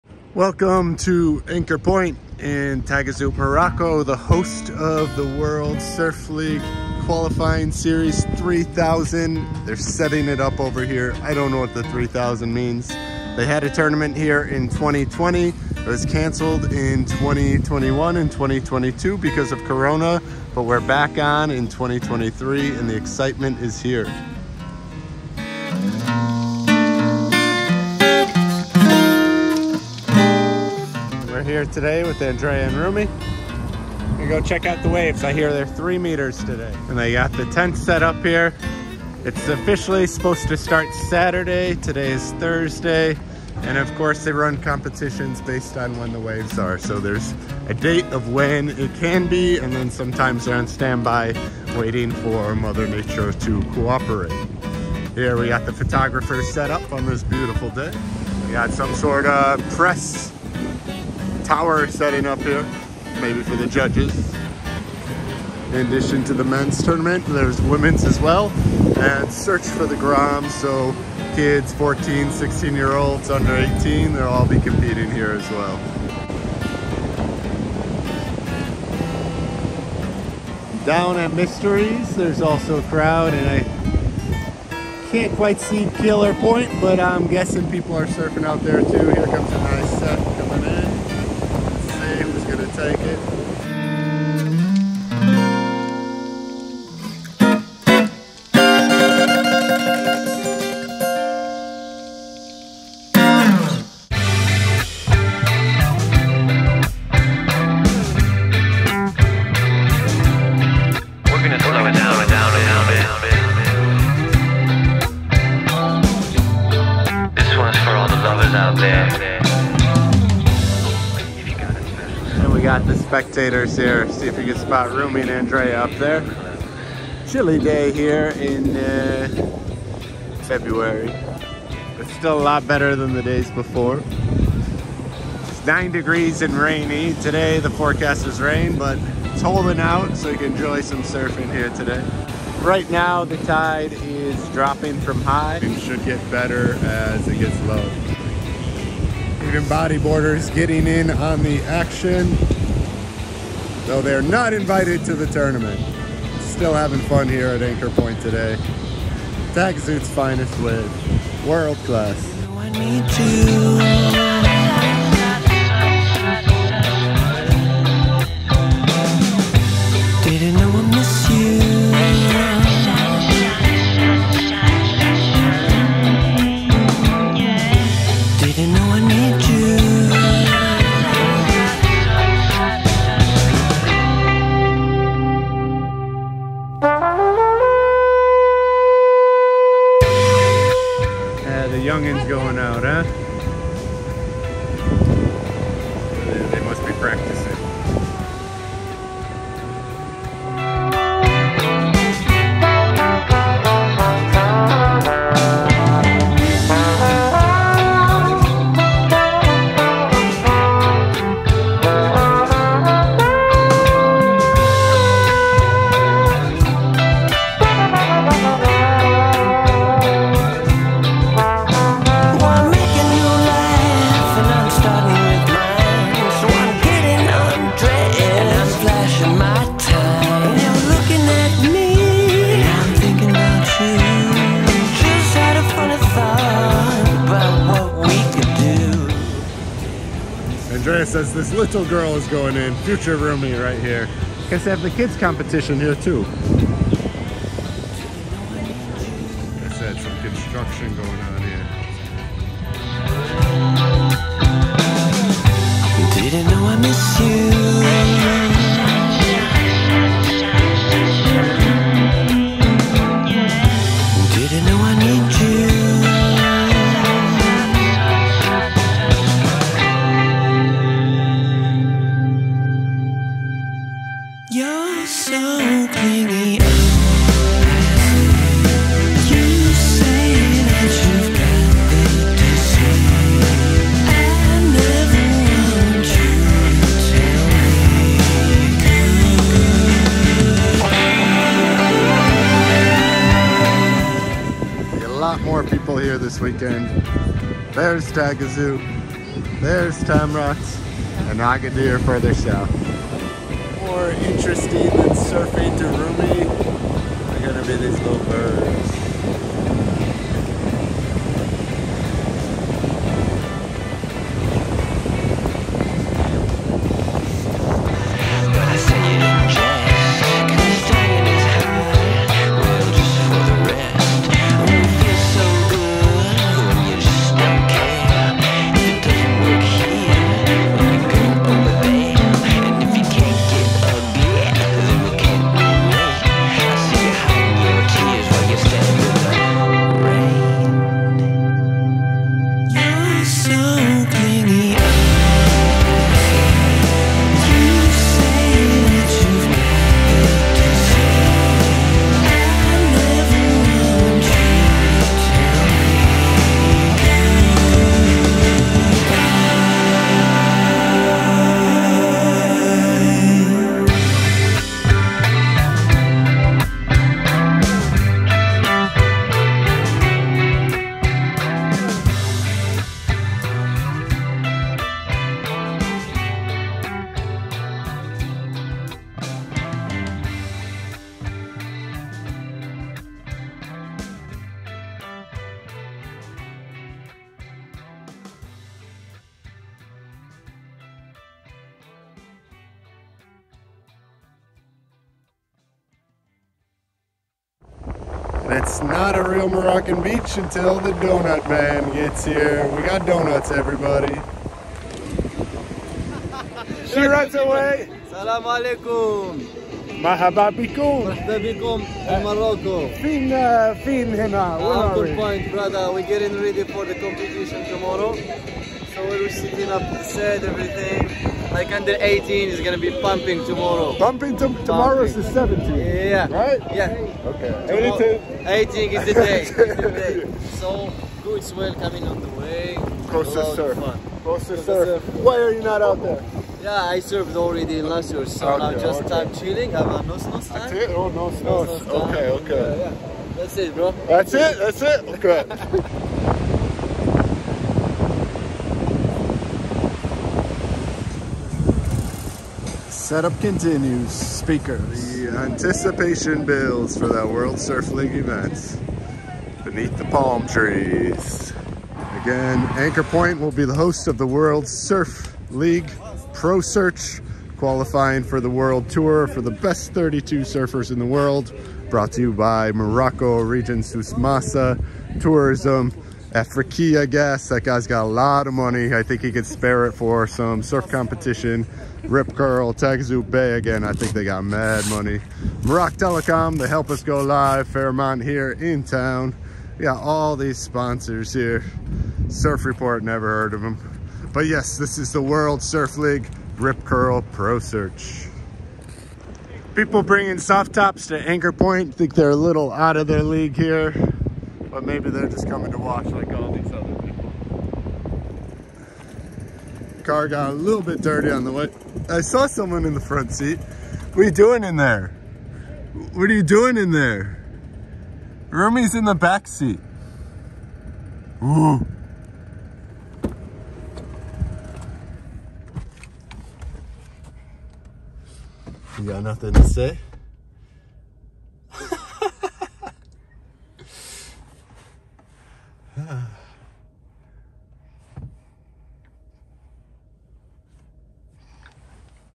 MOROCCO WORLD SURF LEAGUE COMPETITION 🇲🇦 COLD IN AFRICA
Taghazout Morocco hosts World Surf League, or WSL Pro Search in 2023 at Anchor Point. Mor Acro checks in with the location of the WSL pro search contest in Taghazout Morocco, featuring some of the up and coming surfers of all ages, with a special King of the Gromz contest being held. Their is also a womans division added this year and there are Moroccan woman competing for a chance at the national tour. The buzz in Agadir is alive as the Atlantic ocean kicked up 3 meter waves for surfers to warm up on and get acquainted with the spot.

Thanks @kosasurf for the insight to Anchor point also known as Maldraba.

Thanks to the music from @babyjesuspaperboy203